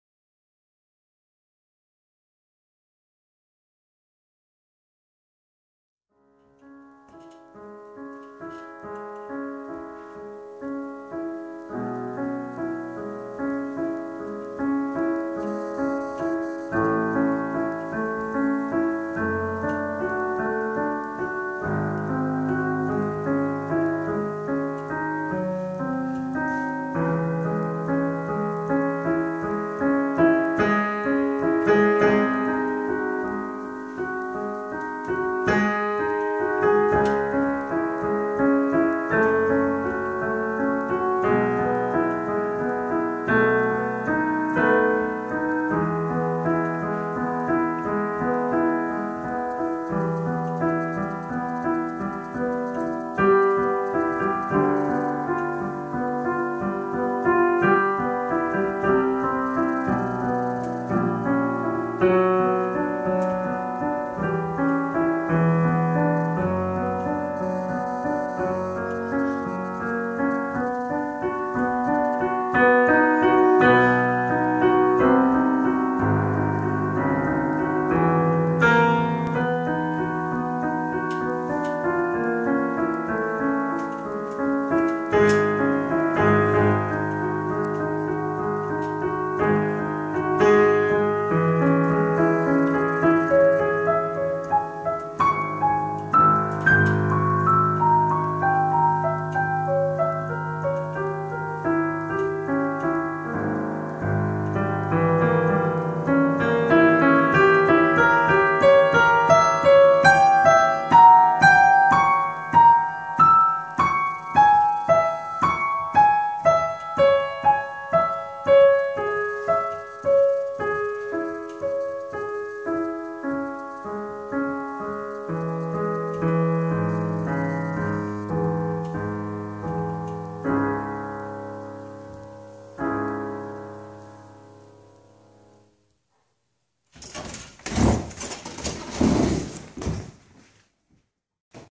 Euan Playing Piano
Euan playing a shortened version of Beethoven's famous tune.